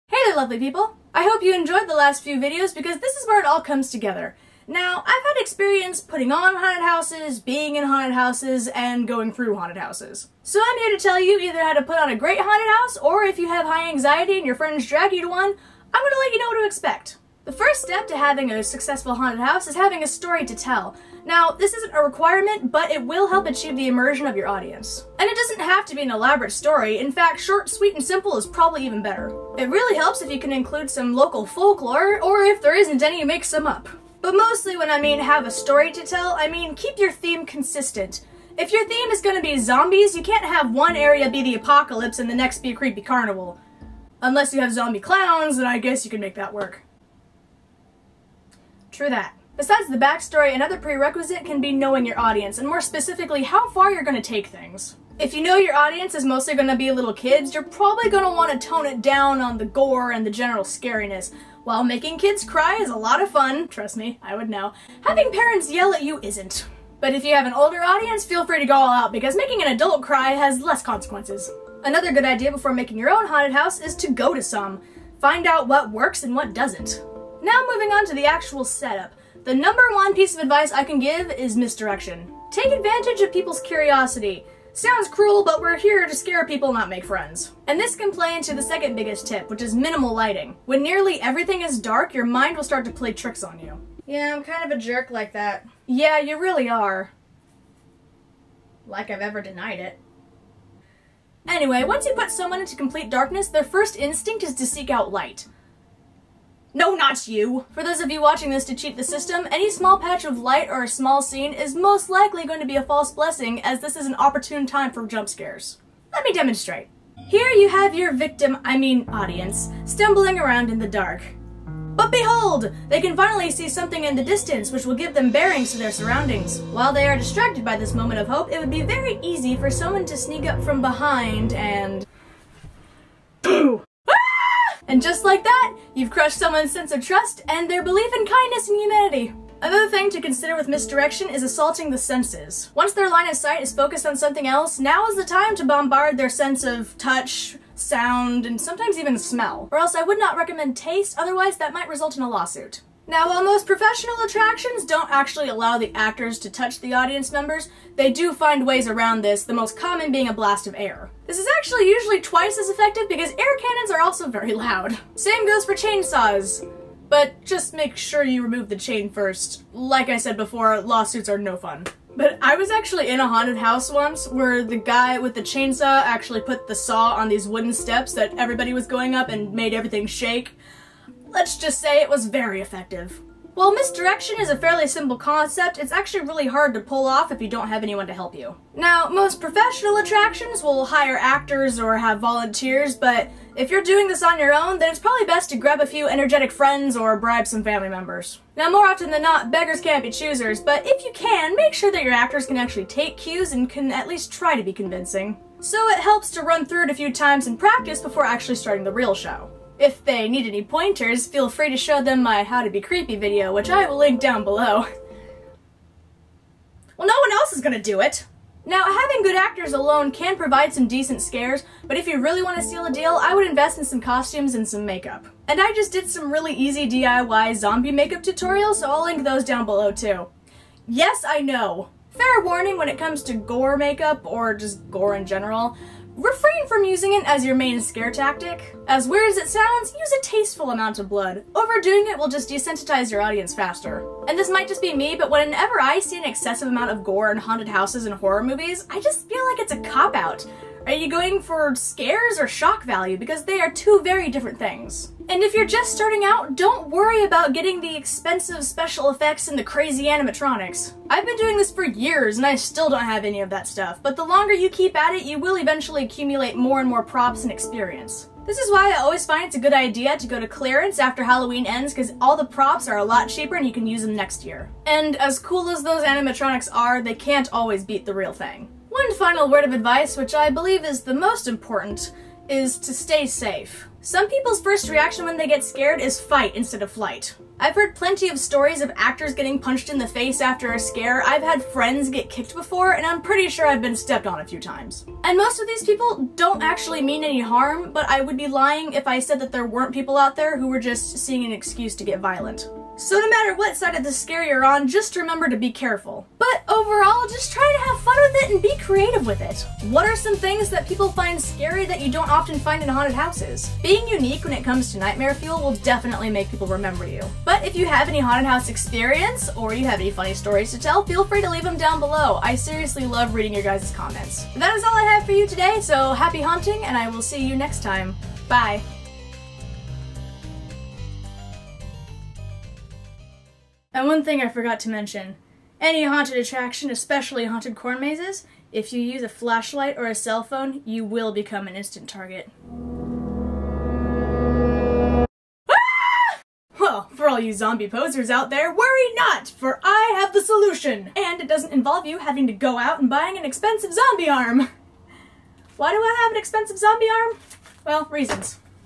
Haunted House Techniques
Are you trying to pull off a super scary haunted house? Or are you nervous about going to one and not knowing what to expect? Either way, I provide you with tips and tricks to help you survive the spooky festivities! And feel free to add your own stories or include any extra advice in the comments below! :D